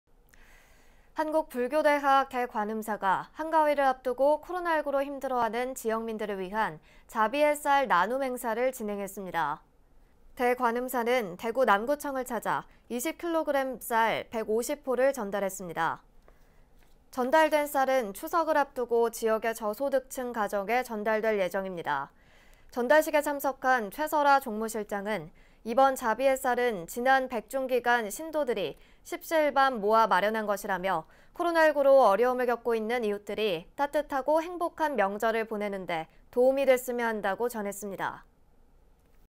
[BBS뉴스] 한국불교대학, 한가위 앞두고 자비의 쌀 전달
한국불교대학 大관음사가 한가위를 앞두고 코로나19로 힘들어하는 지역민들을 위한 자비의 쌀 나눔 행사를 진행했습니다.

大관음사는 대구 남구청을 찾아 20kg 쌀 150포를 전달했습니다.
전달된 쌀은 추석을 앞두고 지역의 저소득층 가정에 전달될 예정입니다.

전달식에 참석한 최설아 종무실장은 “이번 자비의 쌀은 지난 백중기간 신도들이 십시일반 모아 마련한 것”이라며, “코로나19로 어려움을 겪고 있는 이웃들이 따뜻하고 행복한 명절을 보내는데 도움이 됐으면 한다"고 전했습니다.